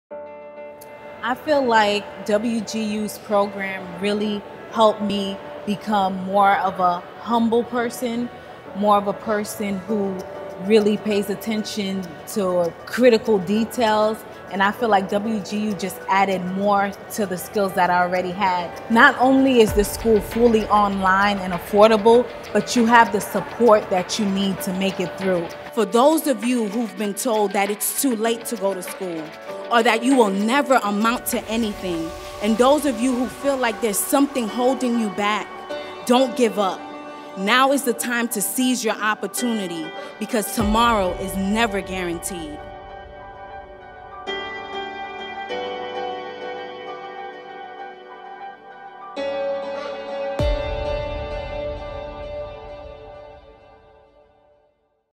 WGU Tennessee Graduate Sabine Laporte - "Now is the time"
Sabine Laporte graduated from WGU Tennessee in 2017 with a B.S., Nursing degree.